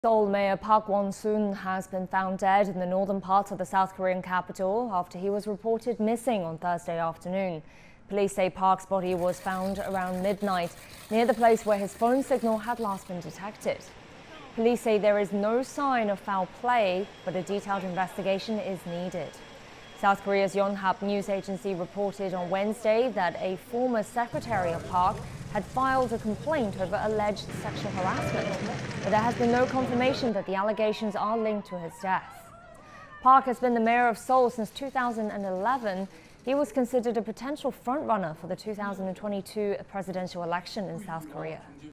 Seoul Mayor Park Won-soon found dead on mountain
Seoul Mayor Park Won-soon has been found dead in the northern part of the South Korean capital after he was reported missing on Thursday afternoon.
Police said Park's body was found around midnight near the place where his phone signal had been last detected. Police also said there is no sign of foul play, but a detailed investigation is needed.